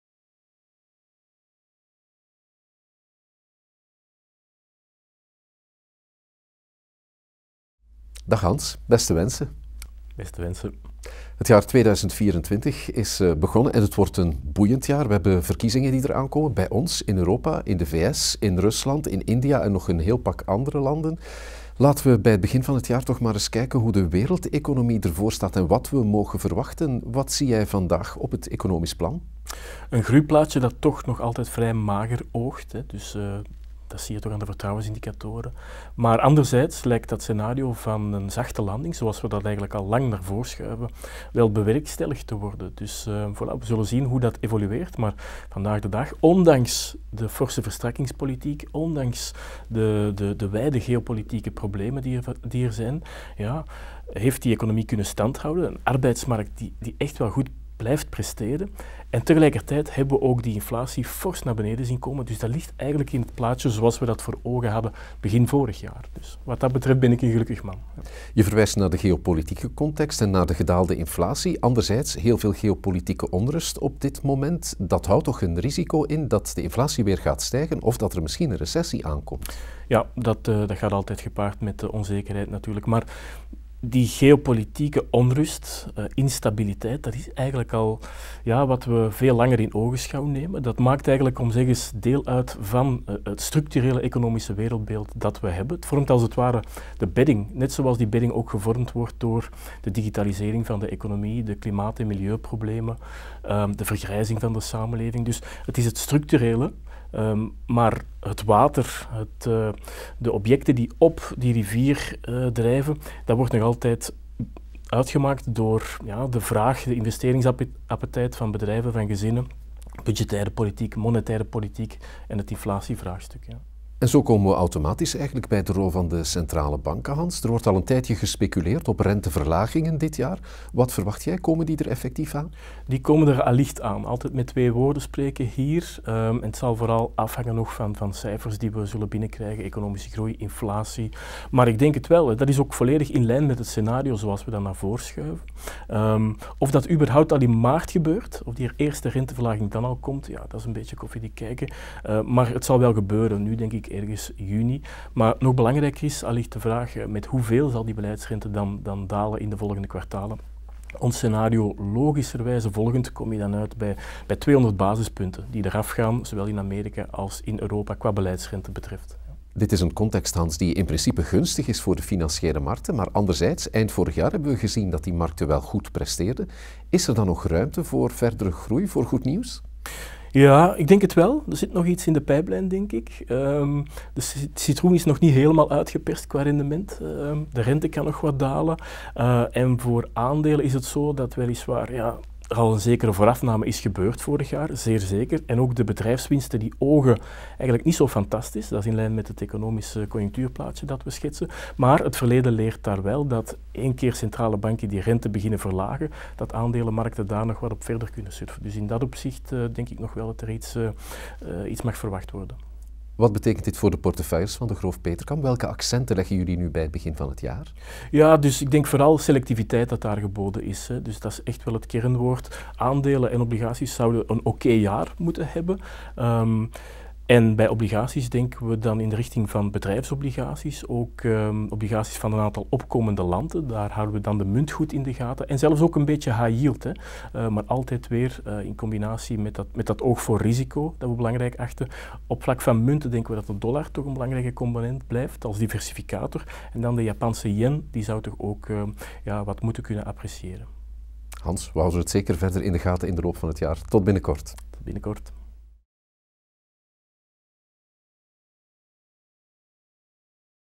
2024, een jaar in het kader van de monetaire versoepeling
Het jaar 2024 is begonnen en het belooft een boeiend jaar te worden. Maar hoe staat de wereldeconomie er momenteel voor? En waaraan mag u zich verwachten? Onze Chief Economist Hans Bevers maakt de balans op en blikt vooruit.

Benieuwd wat Degroof Petercam voor u kan betekenen? Ontdek ons uitgebreide aanbod op